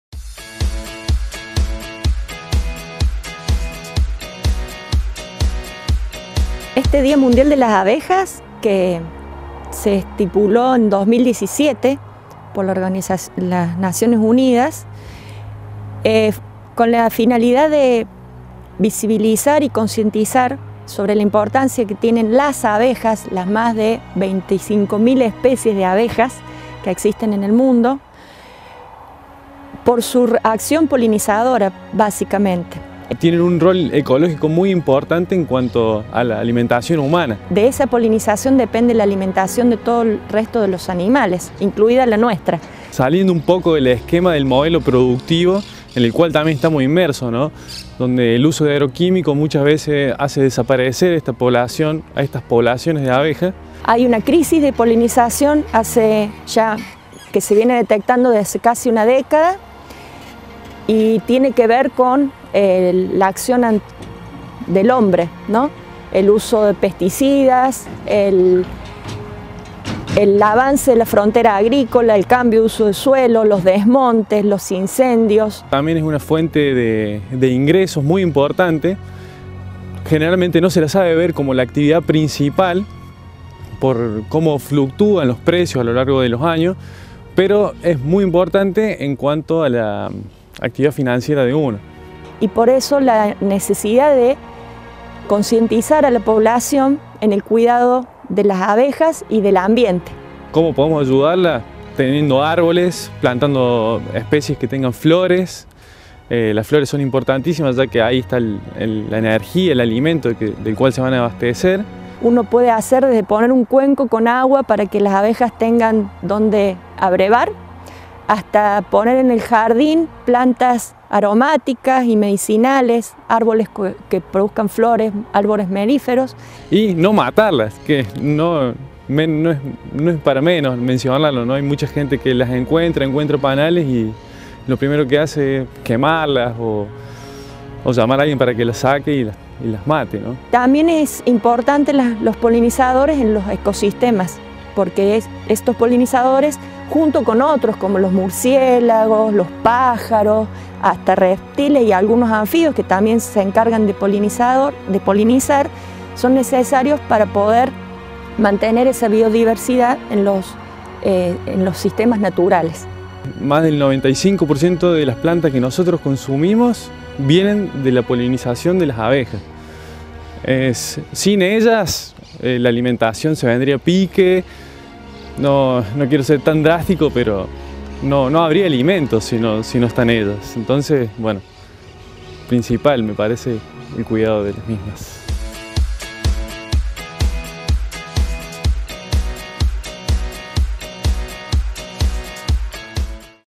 20 de mayo: Día Mundial de las Abejas
Para crear conciencia sobre la importancia de los polinizadores, las amenazas a las que se enfrentan y su contribución al desarrollo sostenible, las Naciones Unidas declararon el 20 de mayo como Día Mundial de las Abejas.

Paula Melegatti - Programa de Apicultura UNRC
Martín Gómez - Estudiantes Ingeniería Agronómica